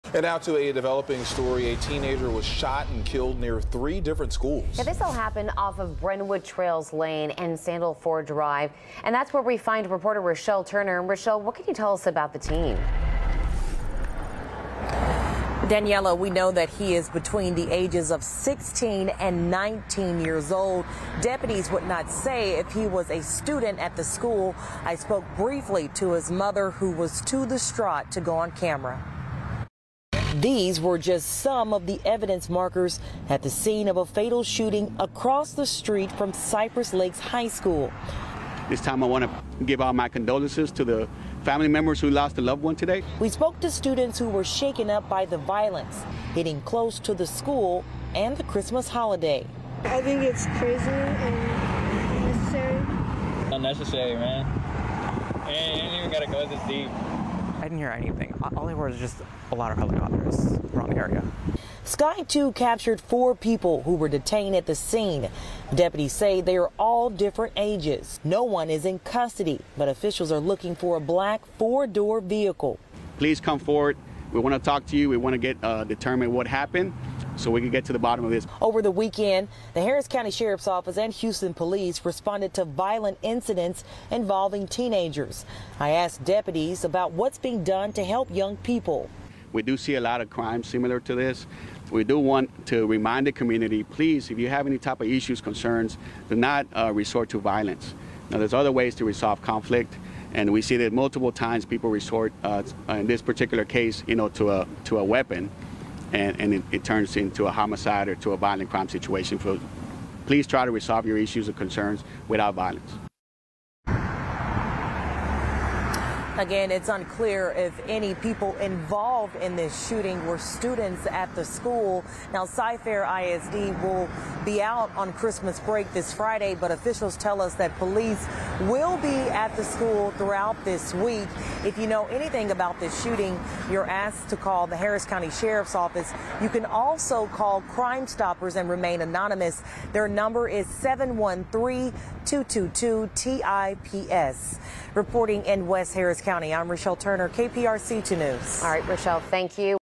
Teen killed in shooting NEAR 3 Cypress-Fairbanks ISD schools
A teen was shot and killed in west Harris County Tuesday, prompting multiple Cypress-Fairbanks Independent School District campuses to go into “Secure the Building” mode, officials said.